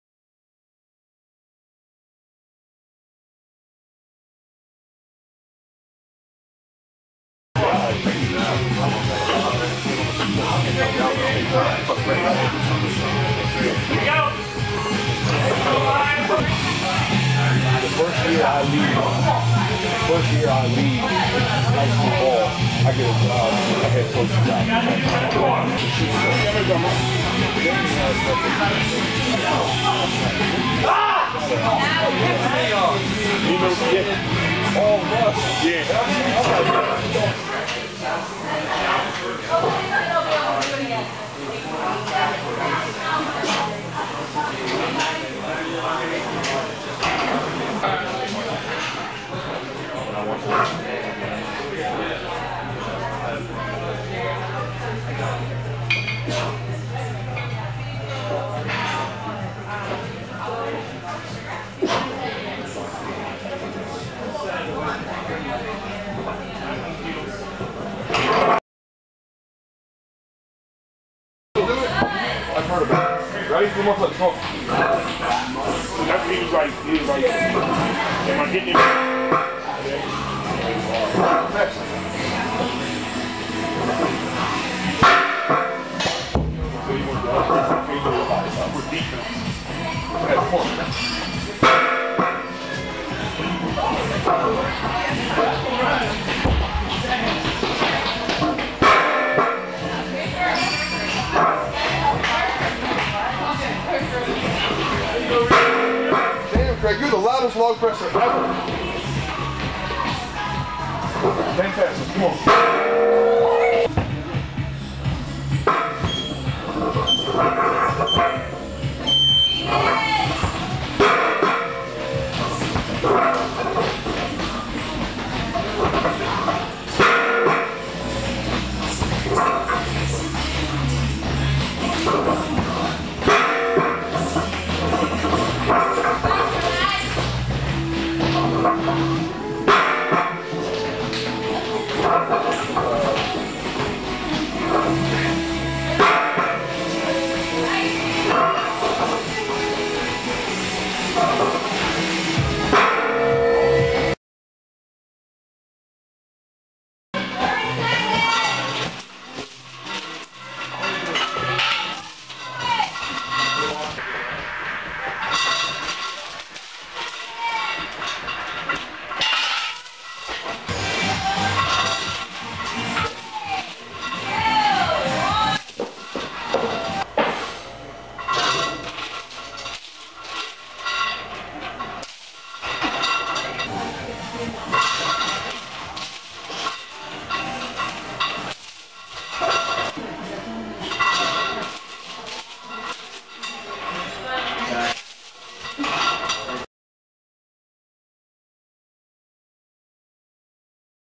Overhead Training - 02/13/2013
Overhead Training Workout for 02/13/2013. Started off with 12" log jerks. Worked up in doubles up to 260x2 and then did 235x6. Followed that up with two sets of 12" log clean and jerks with 205x6. Then six singles of 12" log jerks with chains (40lbs) with 240lbs. Finished up with assistance work for triceps and upper back.